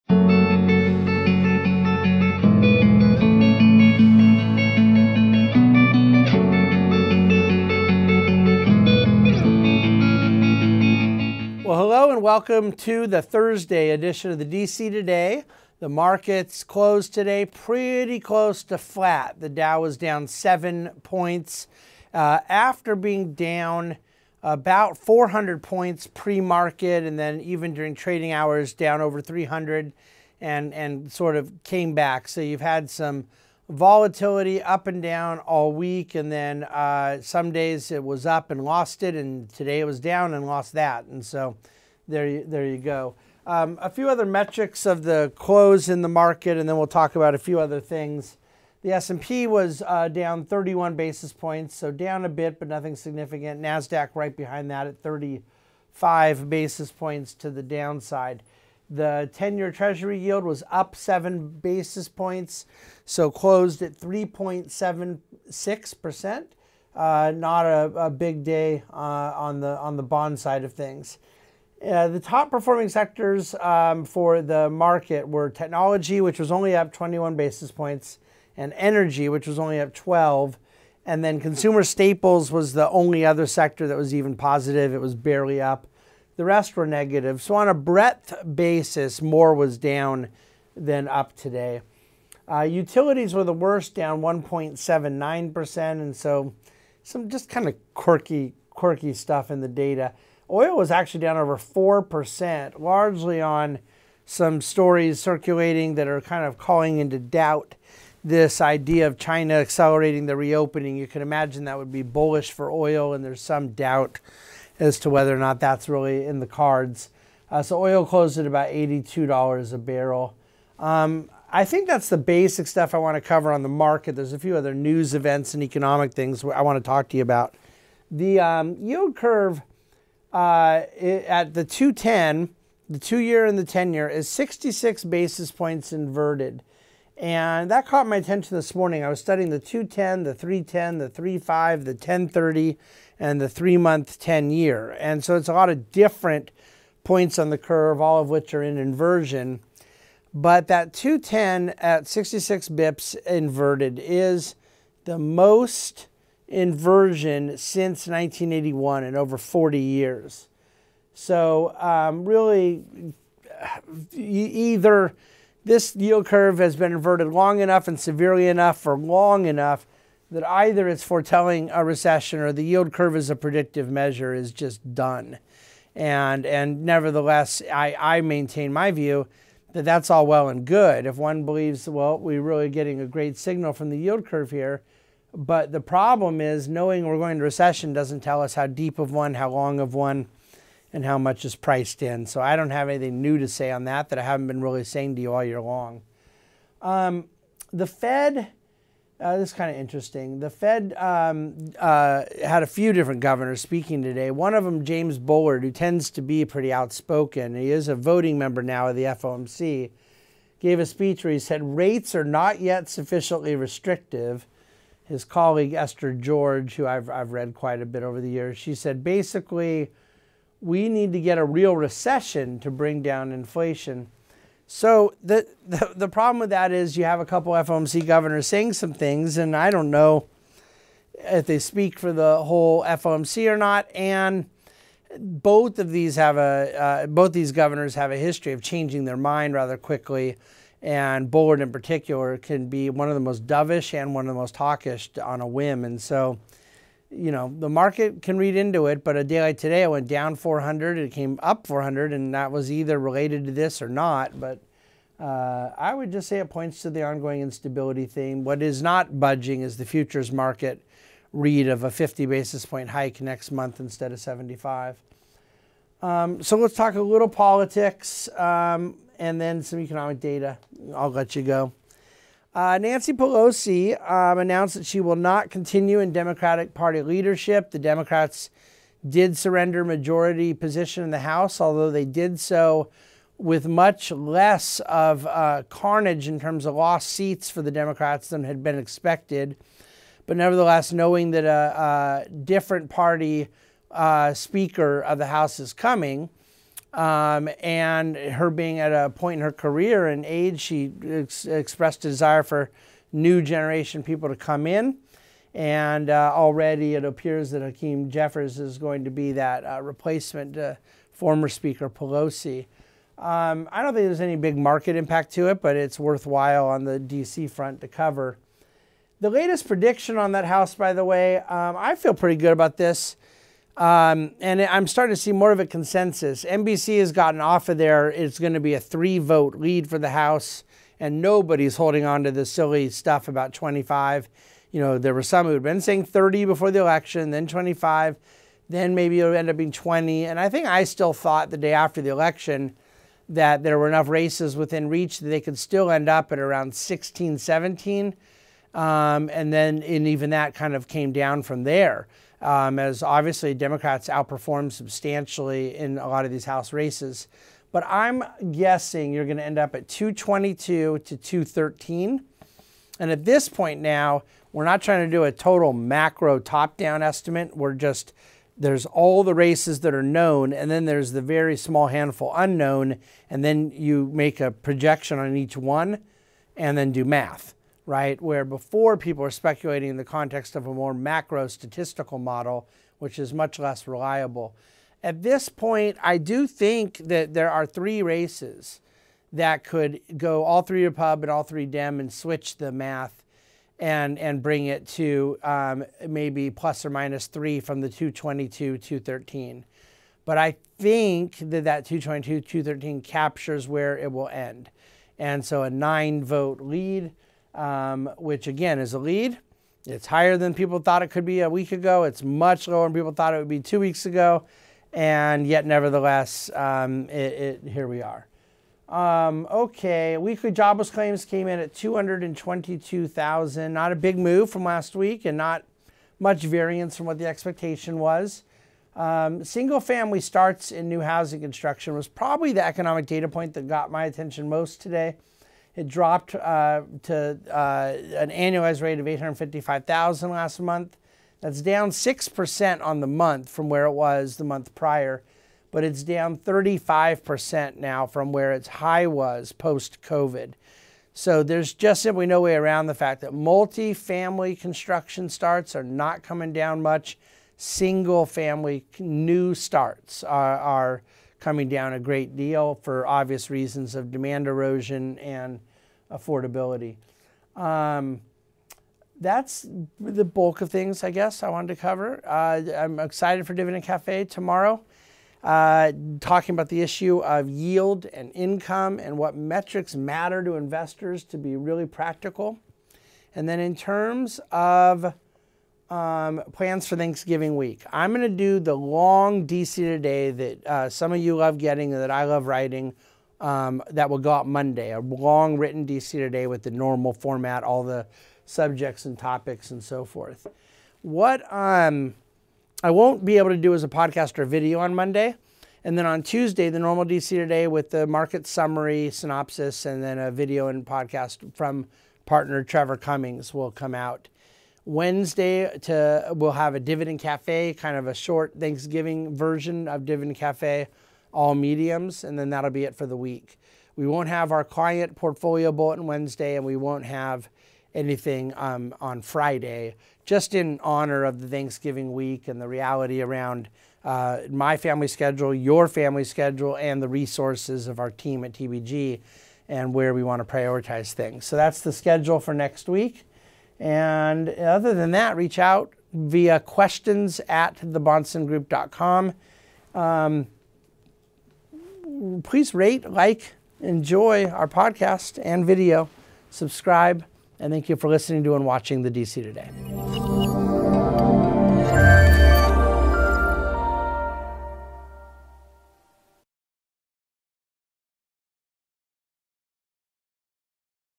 The DC Today - Thursday, November 17, 2022
Another volatile day with the Dow closing flat after being down nearly -400 points.